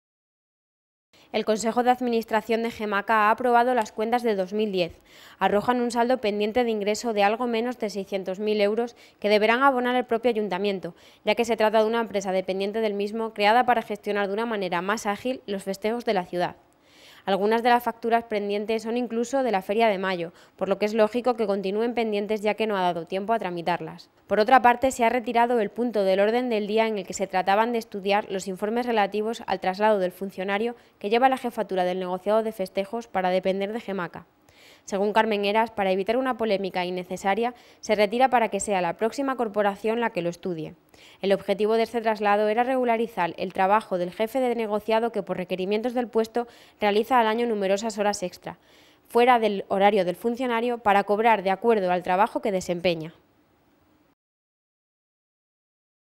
CÁCERES 06/06/11 Empresa Gestora fiestas Cáceres aprueba sus cuentas
El consejo de Administración de Gemaca, la empresa que gestiona las fiestas municipales, ha aprobado las cuentas de 2010.